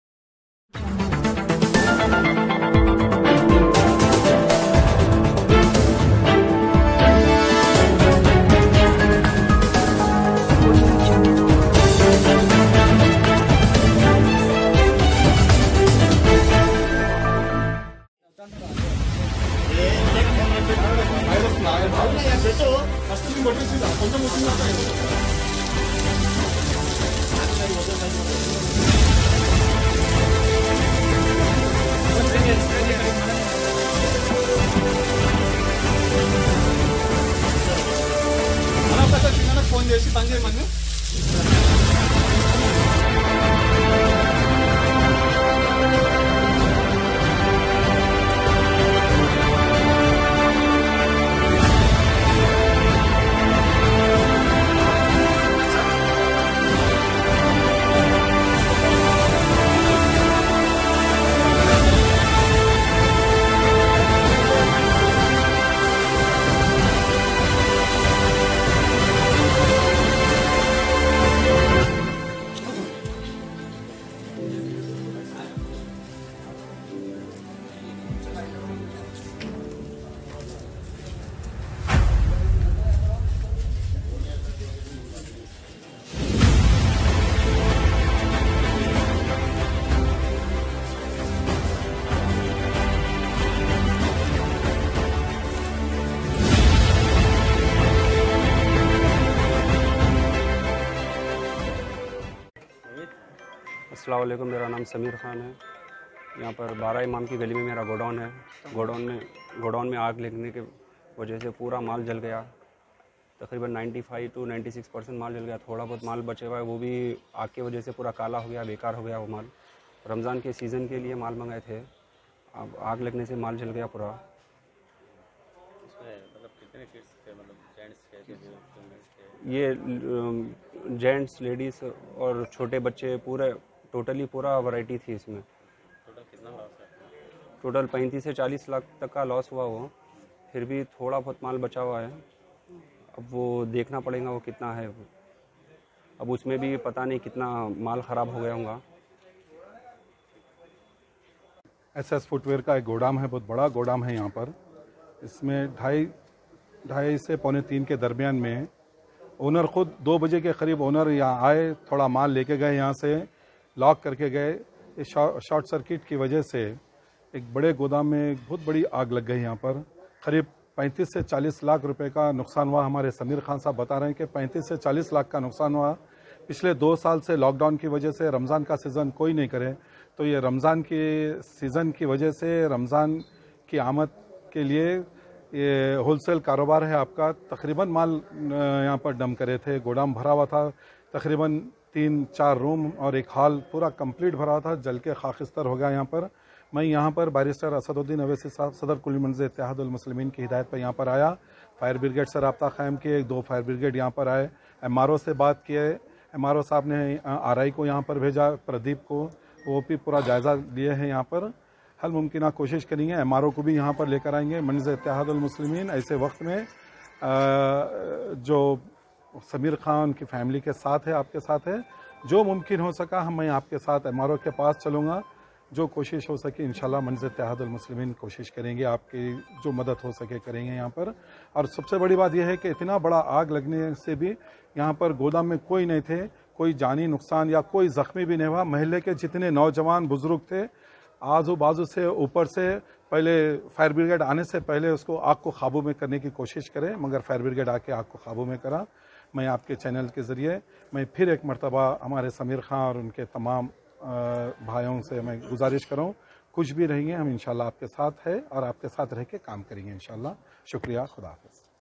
CHAPLO K GODOWN MAI LAGE AAG 40 LAKHS KA NUKSAN AT CHATABAZAR MIM SOHEL QUADRI SAB MOUQE PE MAUJOOD
CHAPLO K GODOWN MAI LAGE AAG 40 LAKHS KA NUKSAN AT CHATABAZAR MIM SOHEL QUADRI SAHAB MOUQE PE MAUJOOD.HYDERABAD